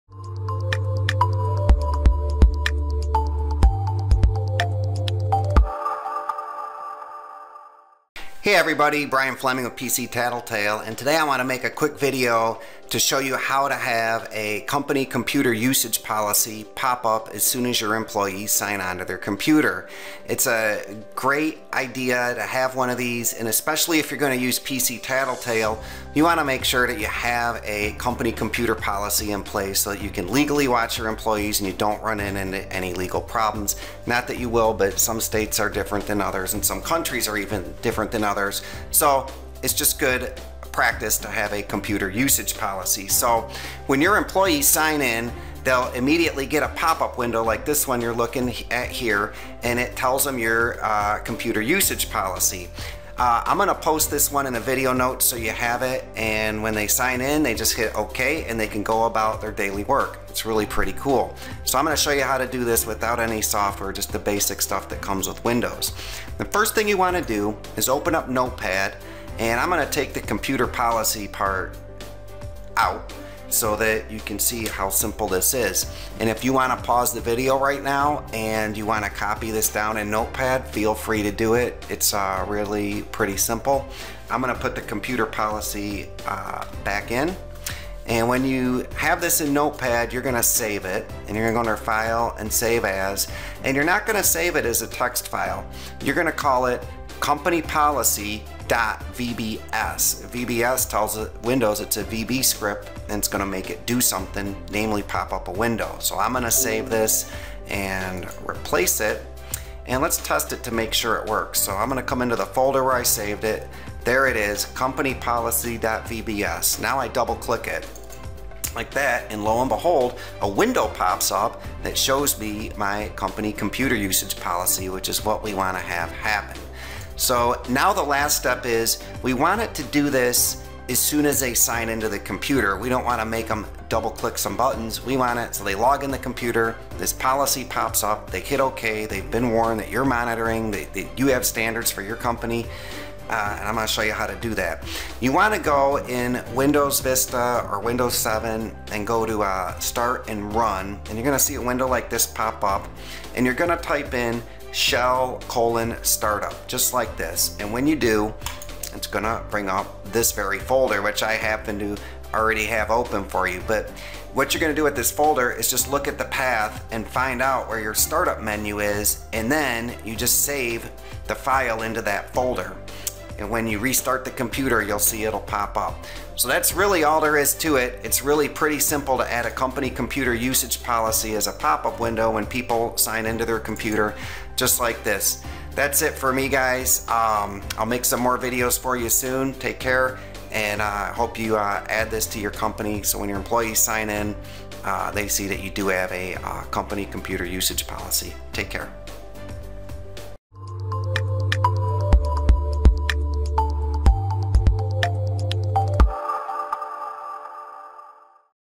Computer Usage Policy For Employees - How to Create One
In this video I show you how to create a simple computer usage policy for your employees.  This policy will pop up on their computer screen the moment they try to log into their computer.

Alll of this can be done with NotePad.  There is no software to buy or use.  It will only take you a few minutes.

Here is a sample computer usage policy that I used in the video:

YOUR COMPANY Name Information and Technology, systems, and their contents are company property.  Employees who use the company's systems are held to the same standard as all other business communications, including compliance with anti-discrimination and anti-harassment, policies, and adherence to copyright laws.  We reserve the right to monitor your activities while using company property